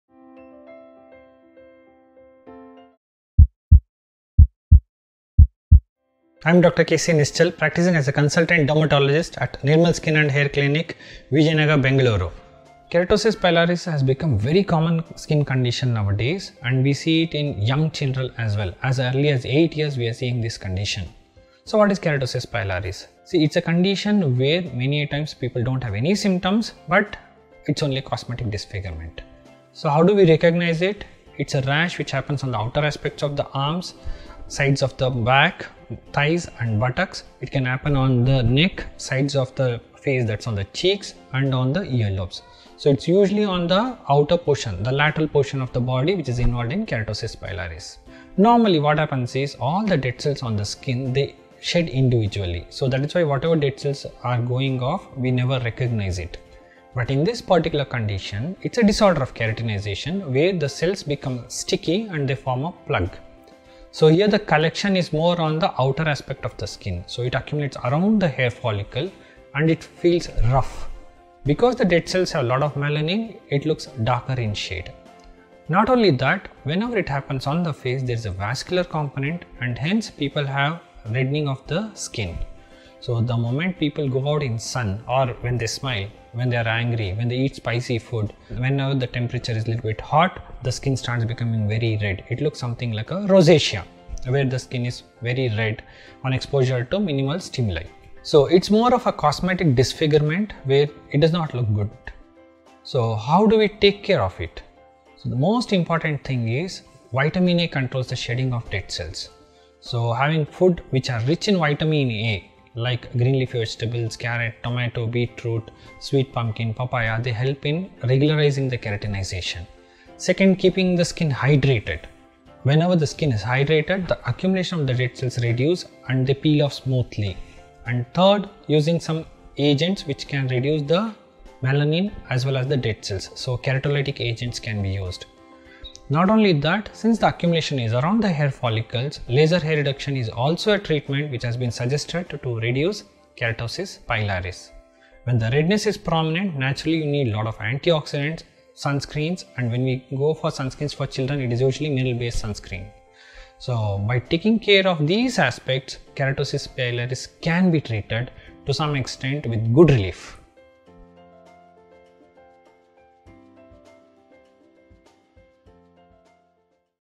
How to get rid of KERATOSIS PILARIS or CHICKEN SKIN expertskisuno -Dr. Nischal K| Doctors' Circle
Consultant Dermatologist & Dermatopathologist | Nirmal Skin & Hair Clinic, Vijayanagar ,Bangalore, India
Keratosis pilaris has become a very common skin condition these days. We see this condition in young children as early as 8 years. How to we recognize it and treat it? To know in detail, watch the video.